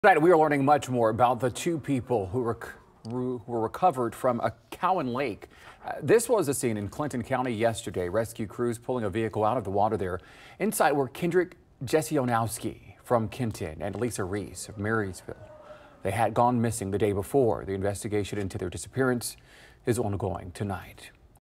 Marysville woman among 2 recovered from truck in southern Ohio lake
The Clinton County Coroner's Office identified the bodies as 50-year-old Kendrick Jesionowski, of Kenton, and 57-year-old Lisa Reiss, of Marysville.